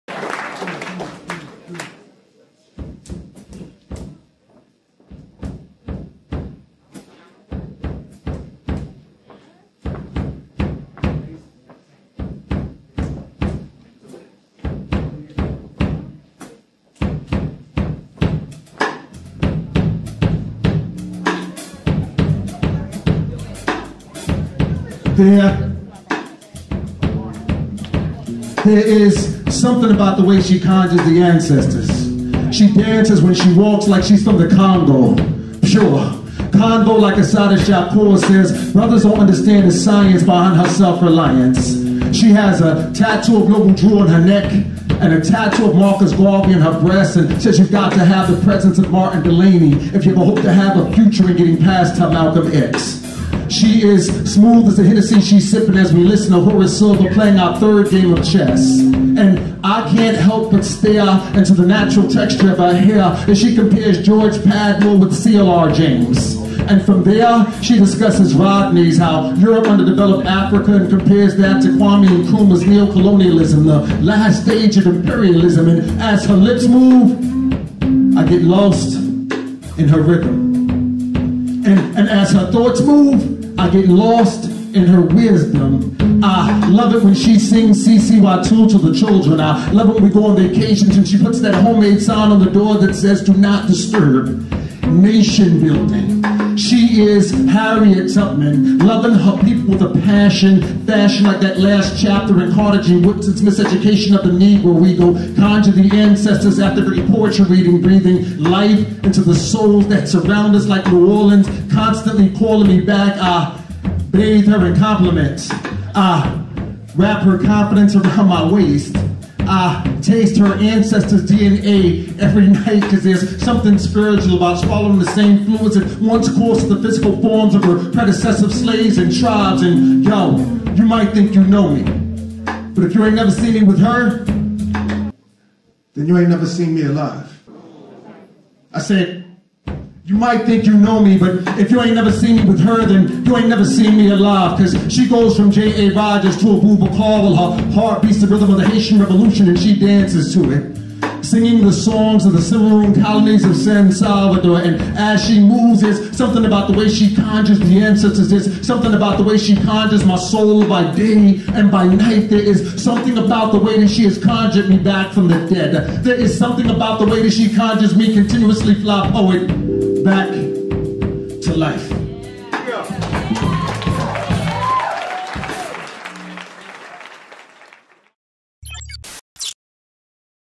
She Conjurez (FlyPoet 2011)
As mentioned early last month, I came across an entire @flypoet feature set from January 2011 that I never got around to watching or, as it would follow, sharing. Since we're all staying-at-home, I'm sharing a piece from the set every few days. This is #4 of 5 - She Conjurez. [Much love to @john.flypoet.hensley who has captured (and selflessly shared) so much of my work throughout the years.] flypoet spokenwordpoetry